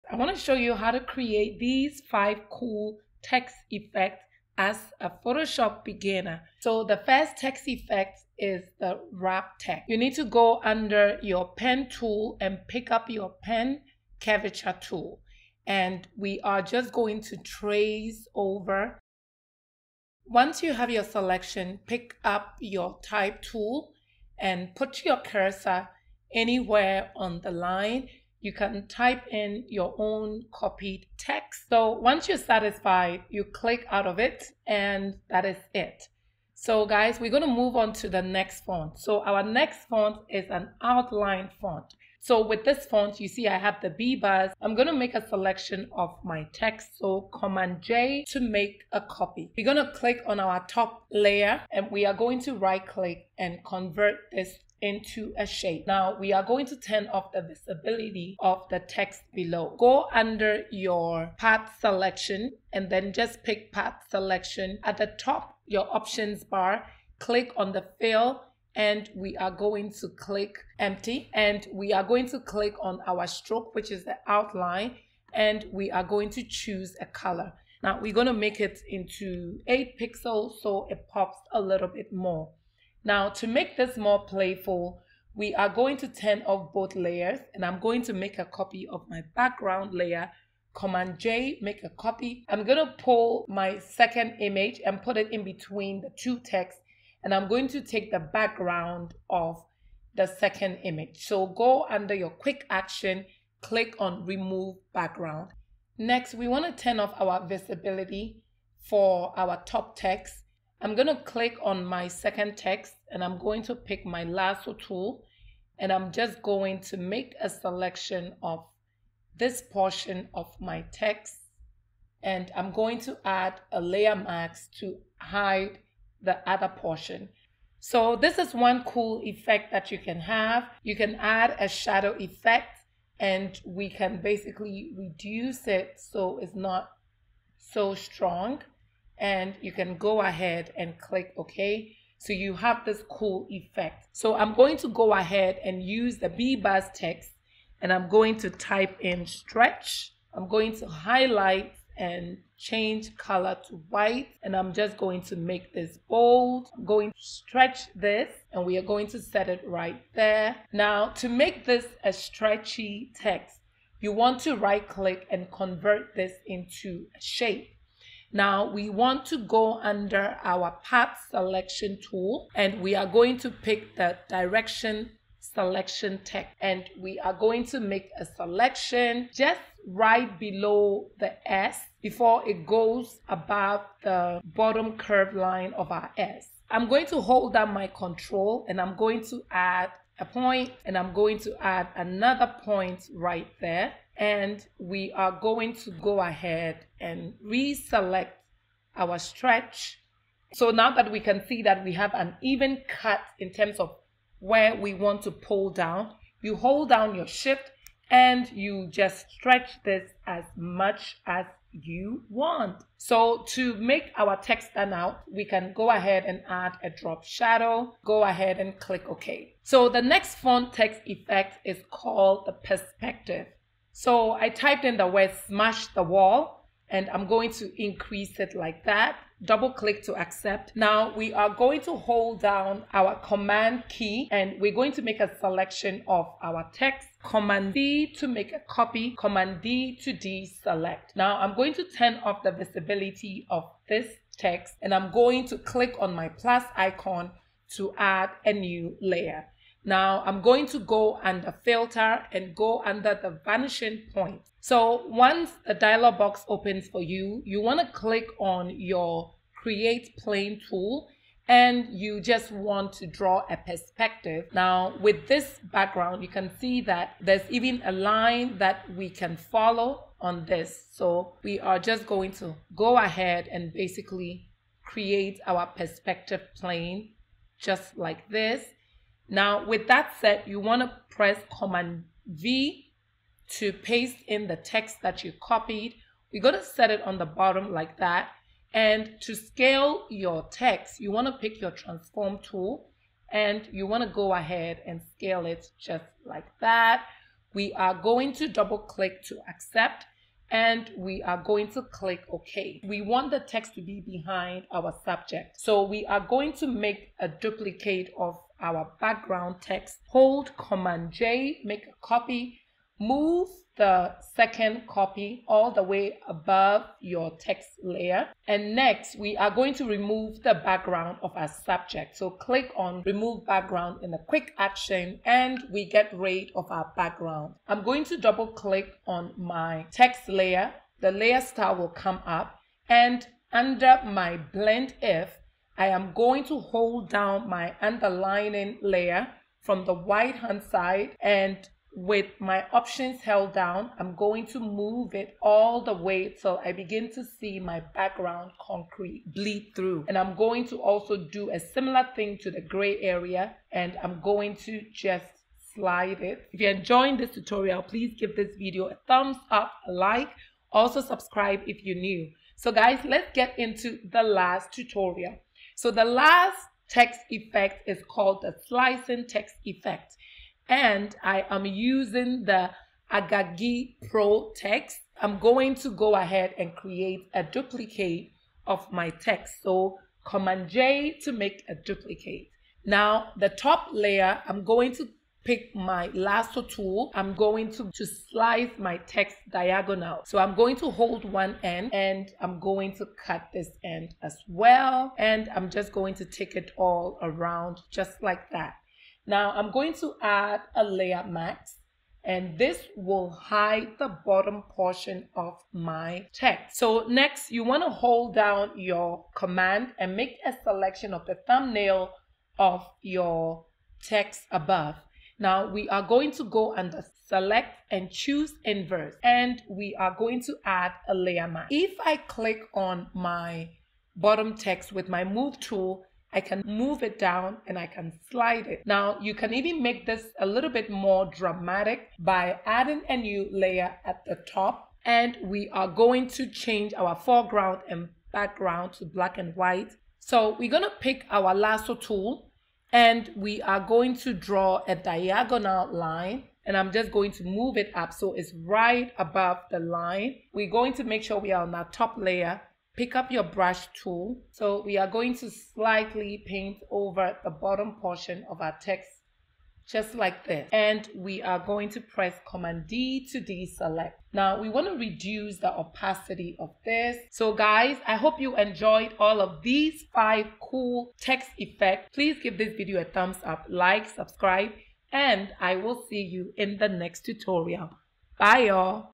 5 Typography Graphic Design Text Effect Ideas | Photoshop Text Effects
Hello everyone, in this photoshop tutorial we will design 5 unique typography design ideas using some very easy steps and methods! These amazing text effects will effortlessly enhance your artwork. From smooth transitions to vibrant color schemes, we'll guide you through practical examples and insider tips.

// B E S T  A D O B E    S O F T W A R E

// M Y  O N L I N E  S T O R E

//F R E E   C O U R S E  -  F O R    B E G I N N E R S

//D O W N L O A D    T U T O R I A L    I M A G E S